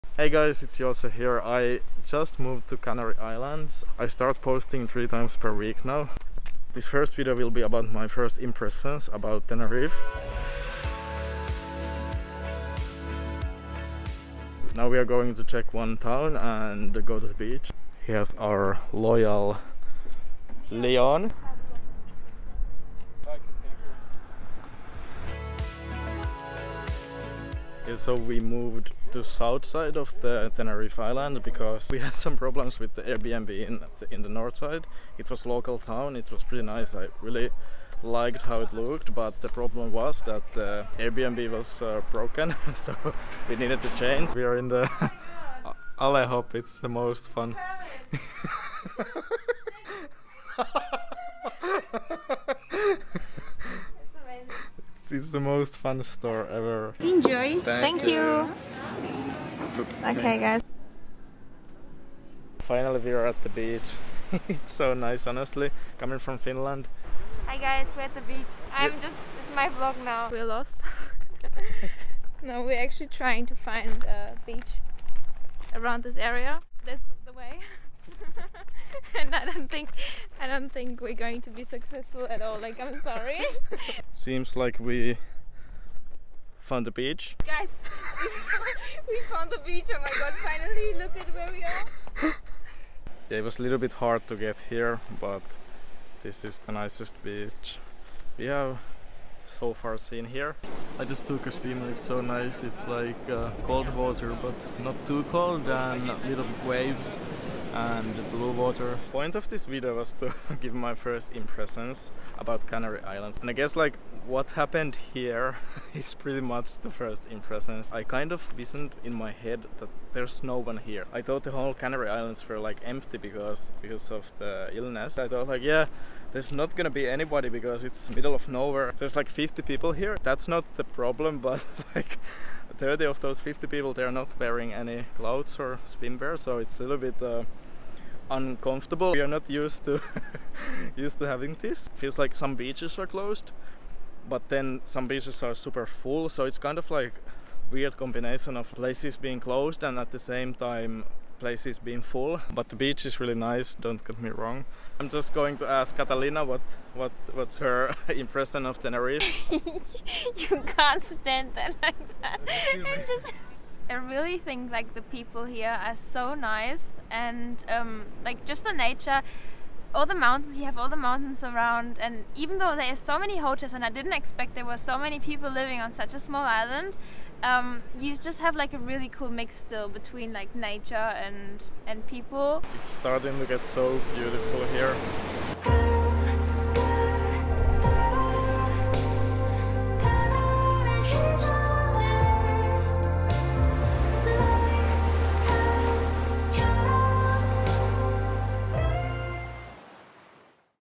Moved to Tenerife (it's not as I expected) | 🇪🇸Canary Islands Vlog 1
Hey guys! In this video, I'm giving my first impressions about Tenerife. Hope you like it!

In March 2021 I decided to move to Tenerife, Canary Islands, Spain. The island living close to the beach with always sunny weather and new places to explore attracted me to Tenerife. I'm having 1-2 months here to test how it is to work remotely here and live the digital nomad lifestyle.